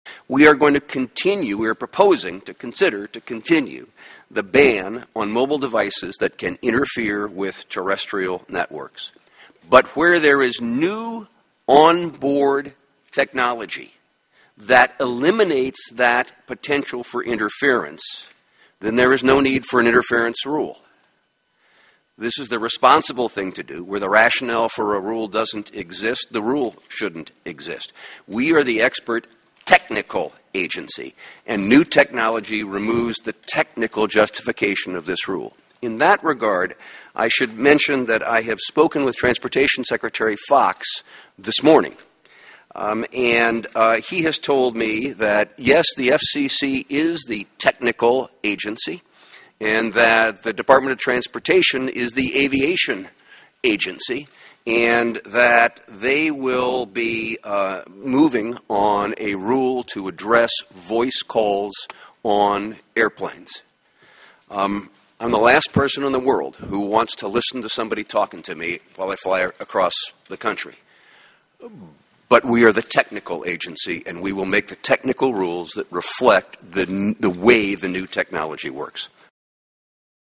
FCC: No Technical Reason to Ban Calls on Planes
Federal Communications Commission Chairman Thomas Wheeler told members of Congress there is no longer a technical reason for the FCC to ban calls on planes, but the Department of Transportation will be moving forward with its own restrictions. (Dec. 12)